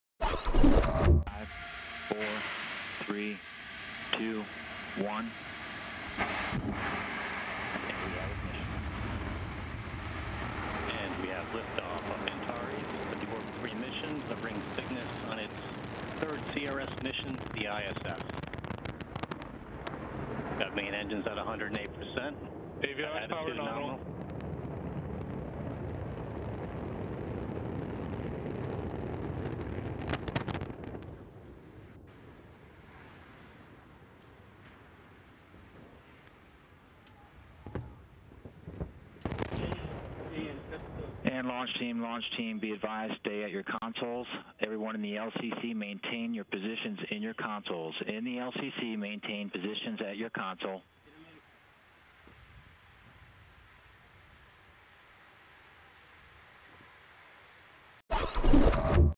Unmanned Antares rocket explodes seconds after take off in Virginia
Mirrored and published with the permission of RT. An unmanned rocket exploded shortly after takeoff Tuesday evening on Virginia’s eastern shore. Orbital Sciences’ Antares rocket was carrying thousands of pounds of equipment to restock the International Space Station.